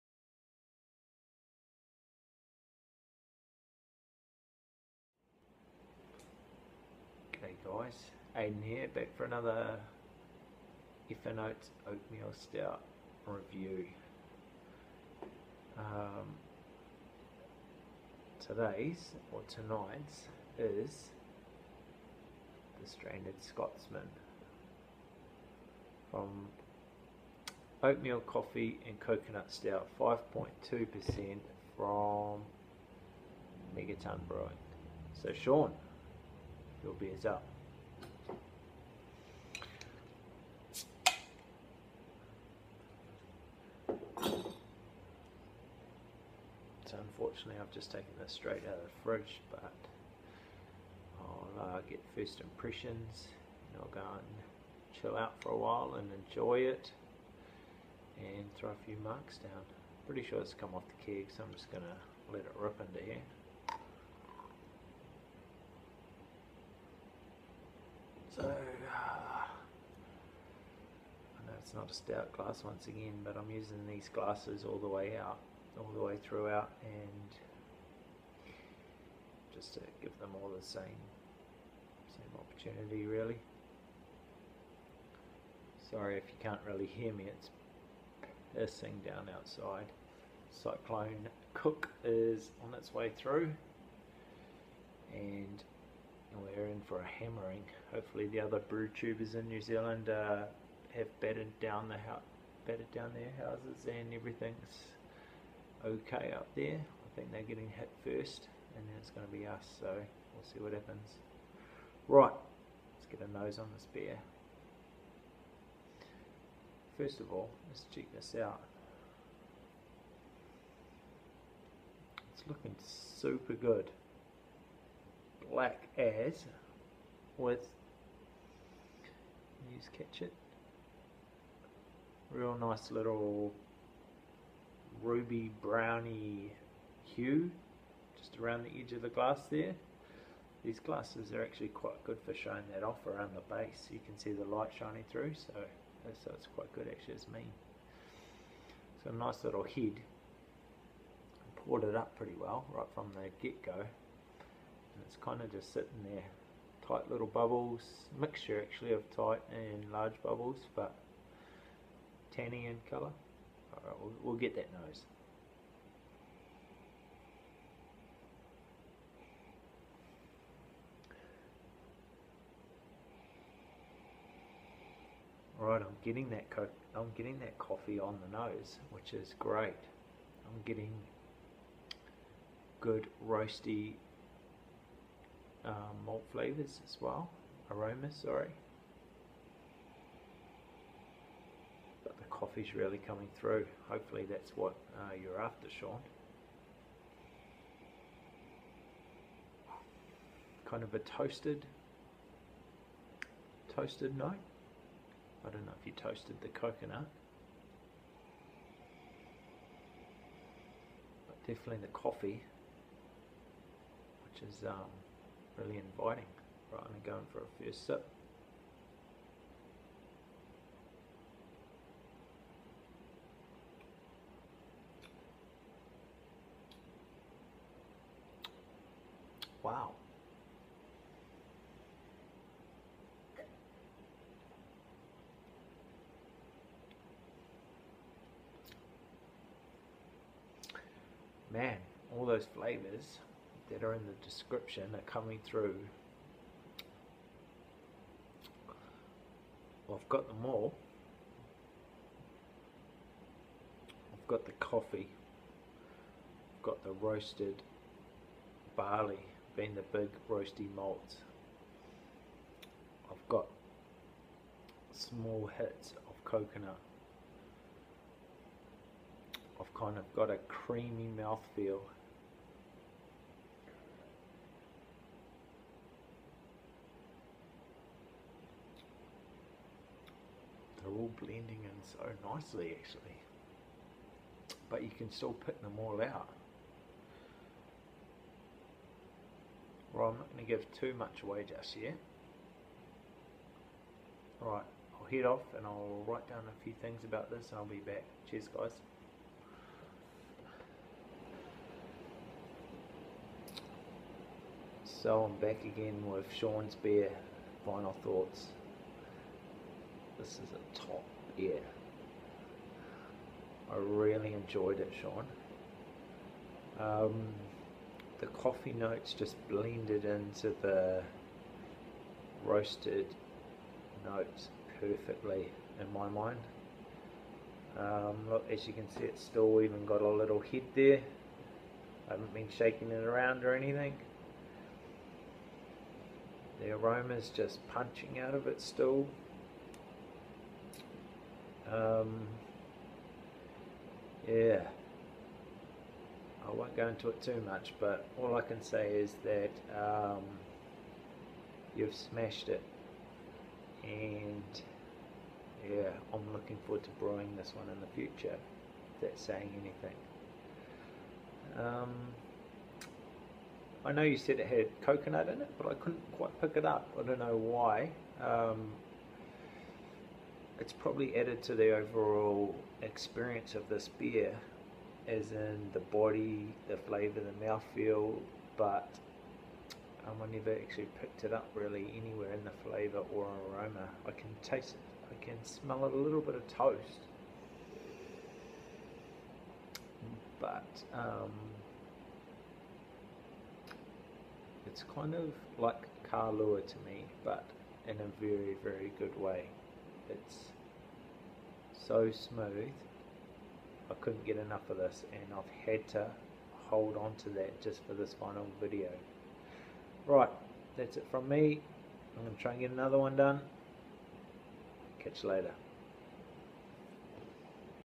F'N Oats Comp beer #3
Oatmeal Stout review from Shaun at 'MEGAtun Brewing' check out his Channel if you haven't alrady!